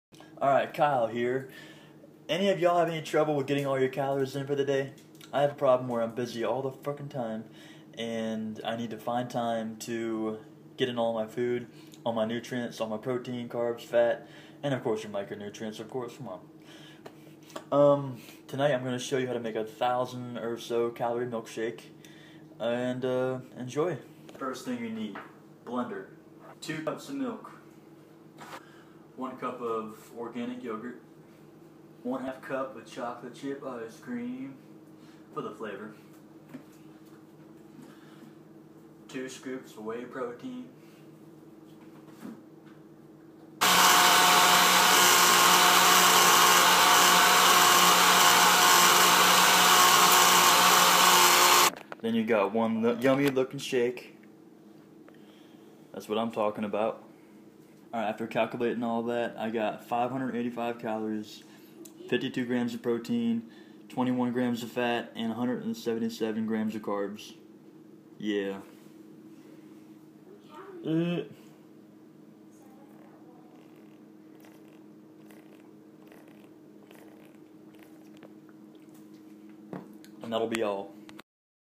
Easy weight gain
Having trouble getting all your calories in time? Just throw everything into a blender.